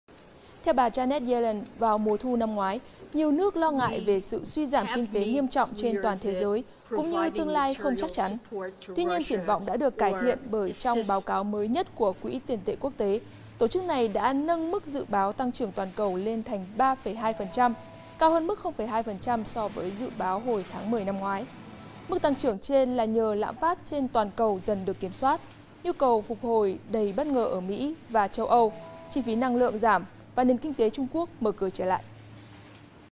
Kinh tế toàn cầu tốt hơn so với dự đoán cách đây vài tháng | VTV4
Triển vọng đã được cải thiện bởi trong báo cáo mới nhất của Quỹ tiền tệ quốc tế, tổ chức này đã nâng mức dự báo tăng trưởng toàn cầu lên thành 3,2%, cao hơn mức 0,2% so với dự báo hồi tháng 10 năm ngoái.

Theo dõi các chương trình của VTV4 tại: Ứng dụng VTVgo trên App store và Google Play hoặc xem trực tiếp trên